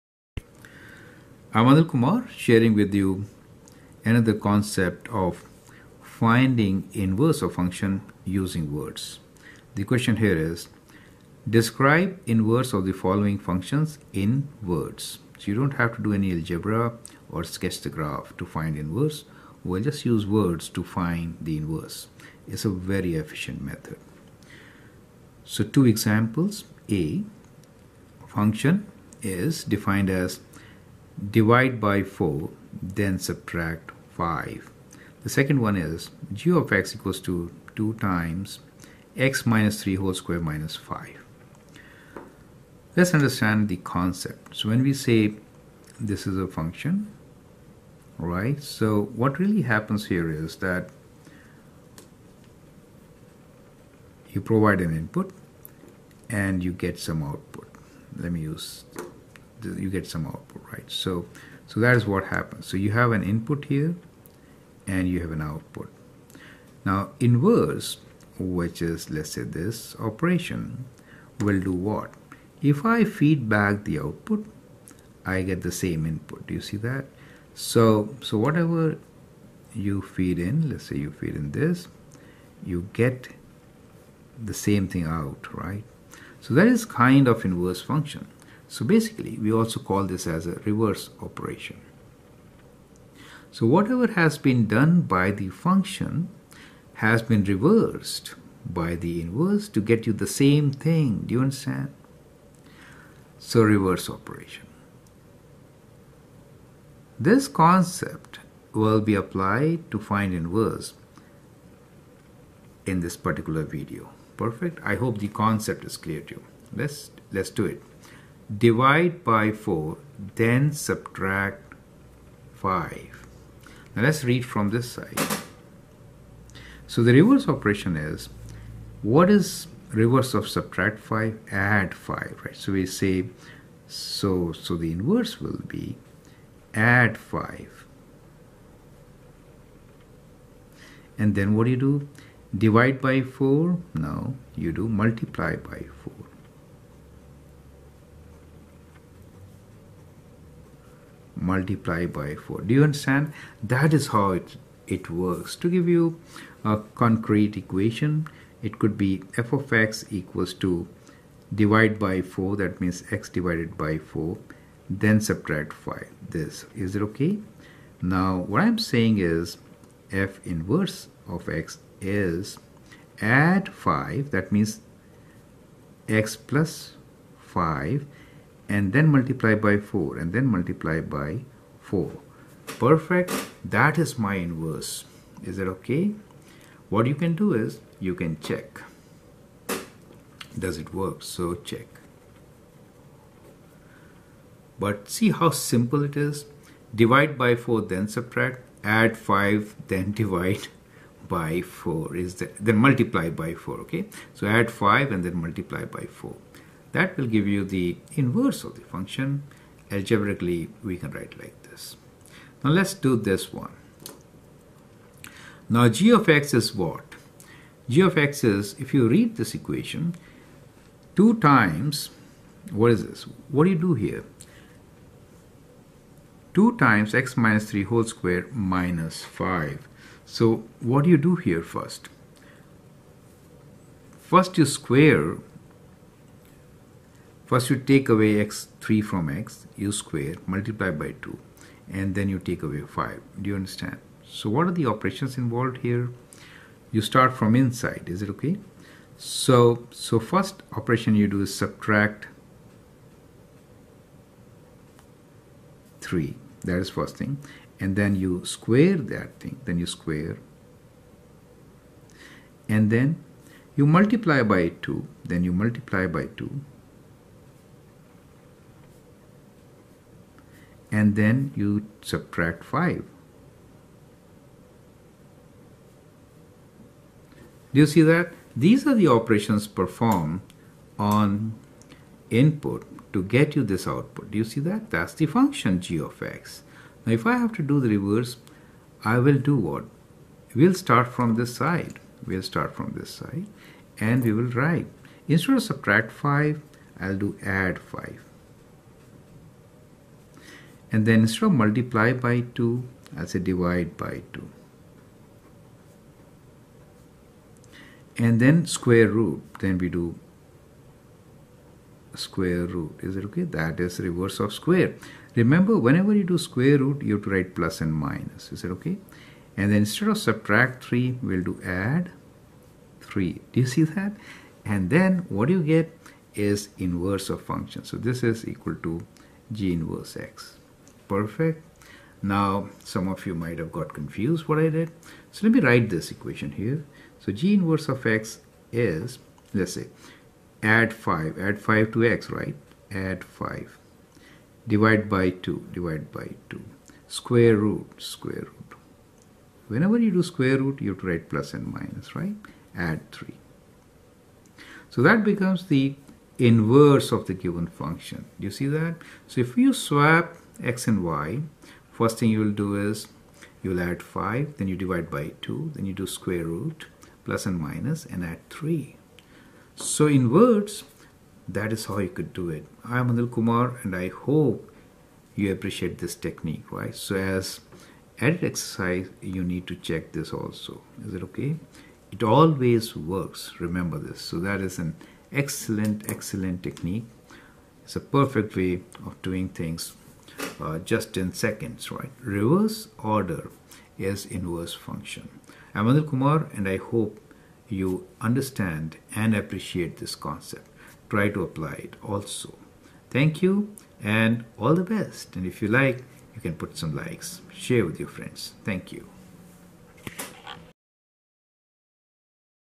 1 How to Describe Inverse Function in Words
If input x gives output y for function, then Inverse function will take y as the input to give x as output. Inverse is not reciprocal, and inverse may not always be a function.